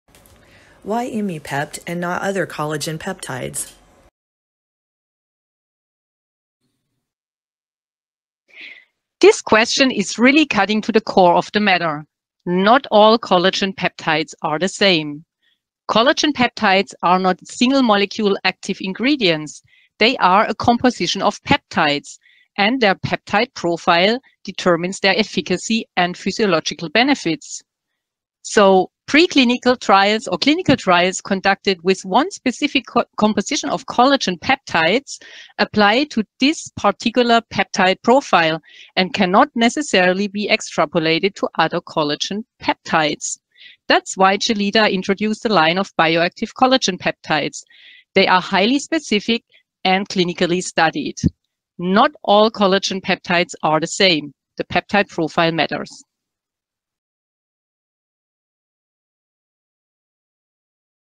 Why not other Collagen Peptides?
In this short video our colleague explains, why not all Collagen Peptides are the same and why the Peptide profile matters!